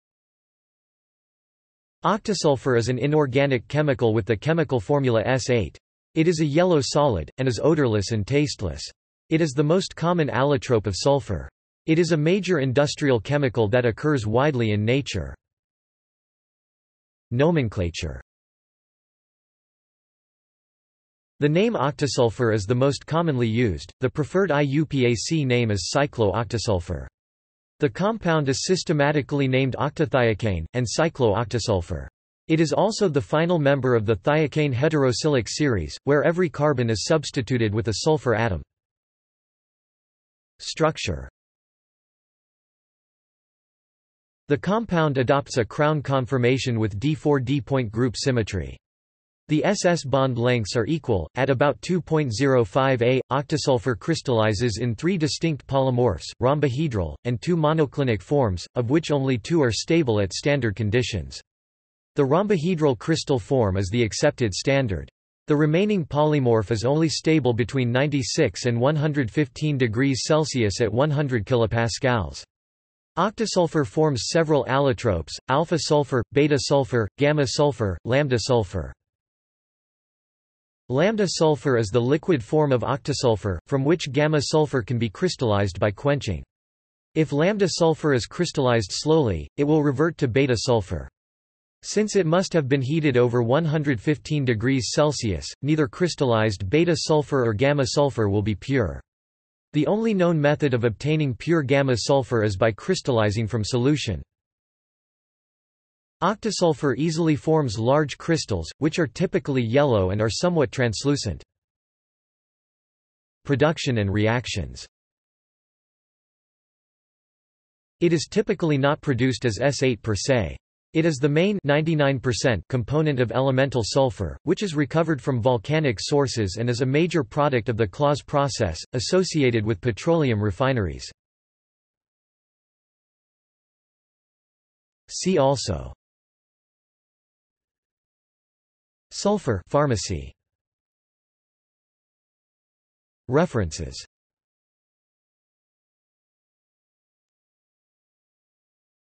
Octasulfur | Wikipedia audio article
This is an audio version of the Wikipedia Article:
Octasulfur

SUMMARY
Octasulfur is an inorganic chemical with the chemical formula S8. It is a yellow solid, and is odourless and tasteless. It is the most common allotrope of sulfur. It is a major industrial chemical that occurs widely in nature.